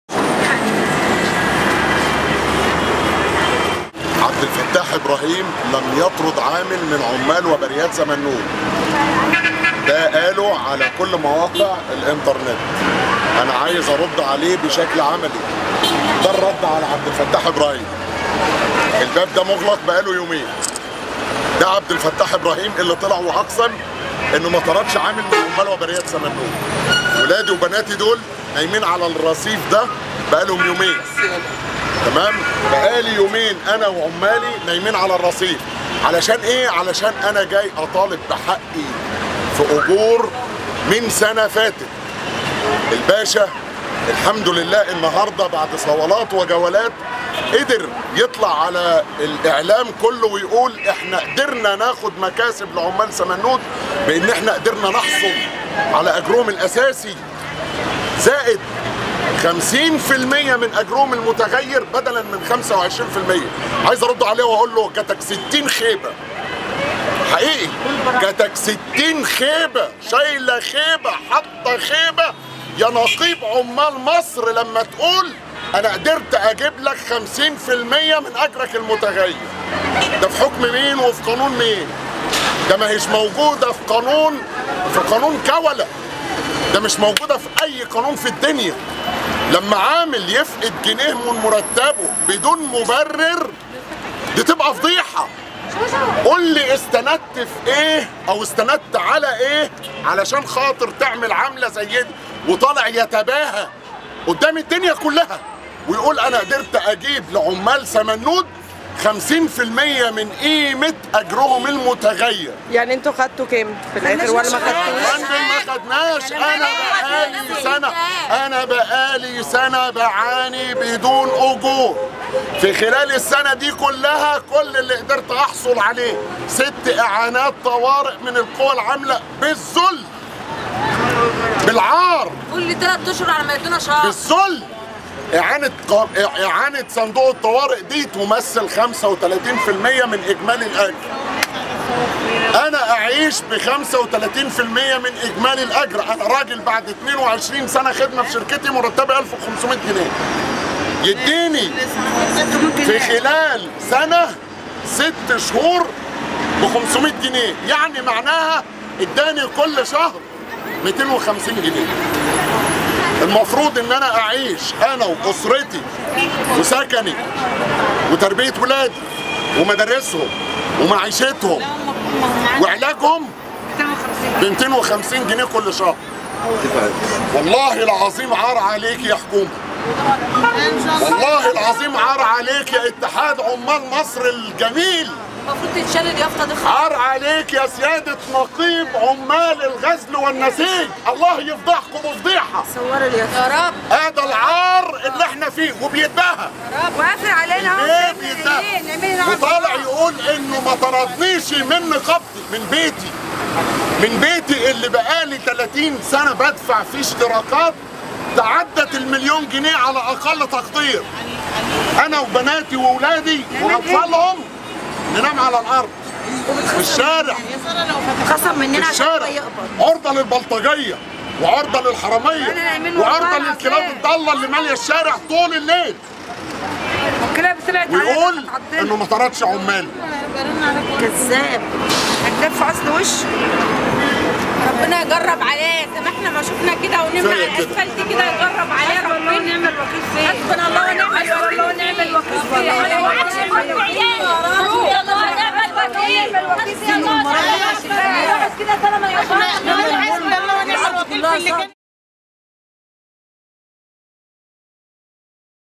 عمال «وبريات سمنود» 11 شهر بدون أجور | تلفزيون الاشتراكي
فض أمس عمال سمنود اعتصامهم الذي بدأ يوم الأحد أمام مقر النقابة العامة للغزل والنسيج بشبرا بعدما أغلقت في وجوههم أبواب النقابة، وكان قد توجه وفد من العمال إلى القاهرة لمقابلة وزيرة القوى العاملة “ناهد العشري” بمقر النقابة العامة للتفاوض بشأن رواتبهم المتوقفة منذ 11 شهرًا وإعادة تشغيل الشركة.

تصوير واعداد جيهان ابراهيم